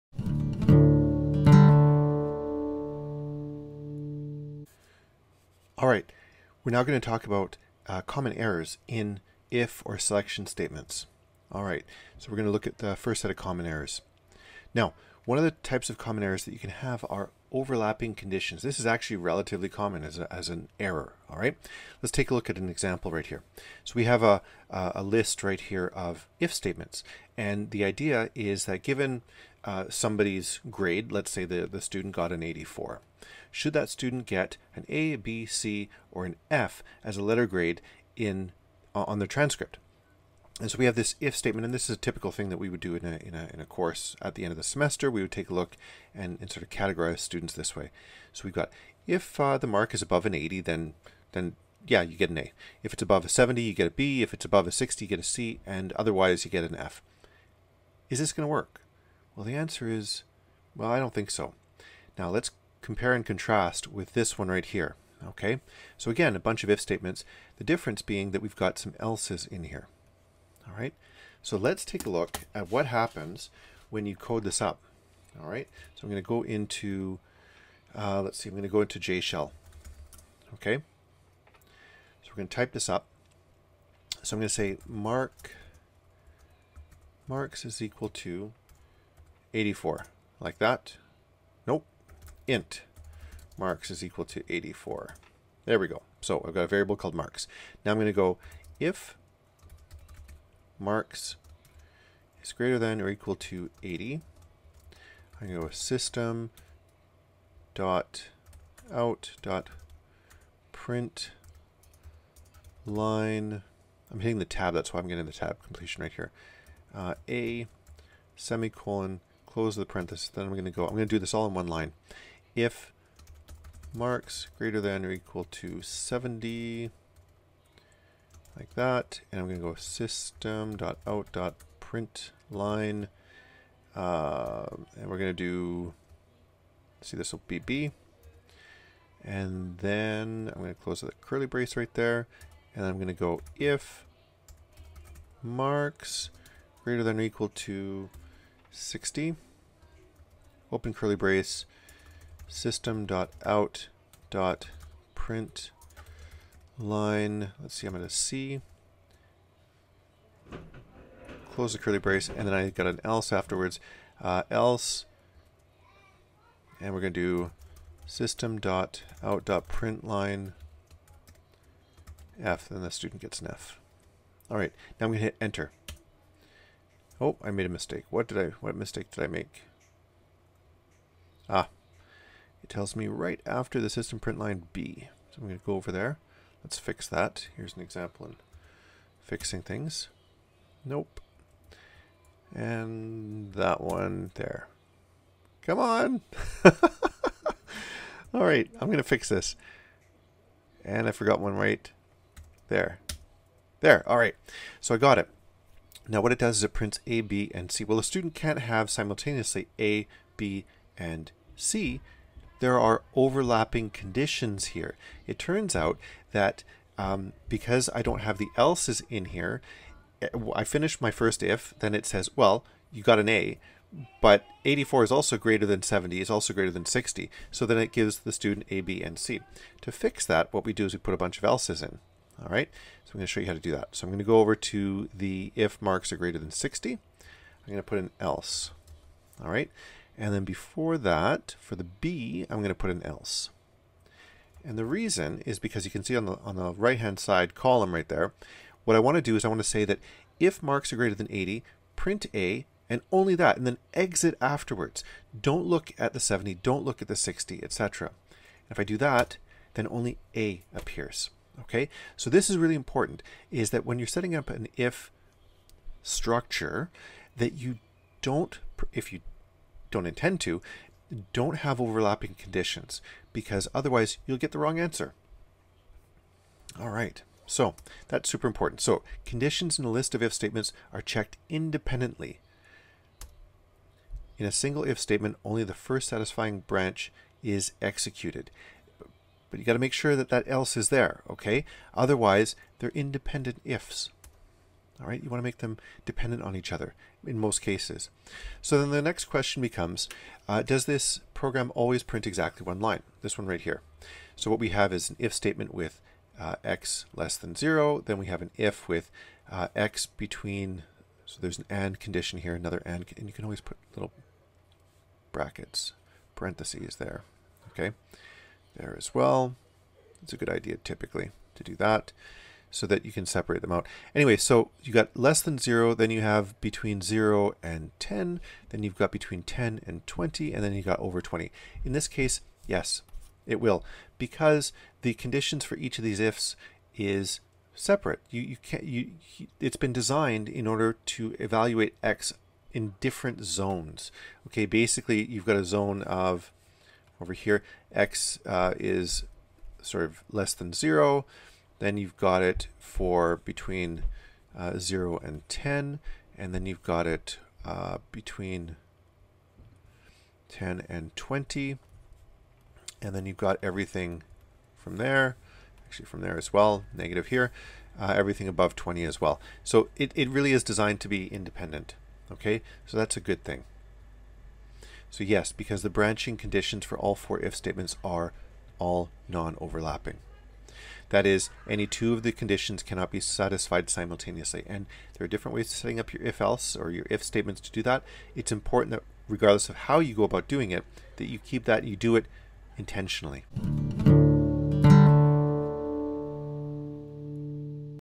Class: Selection Statements in Java -- Common Errors (Overlapping Conditions)
Here I show what overlapping conditions looks like using jshell.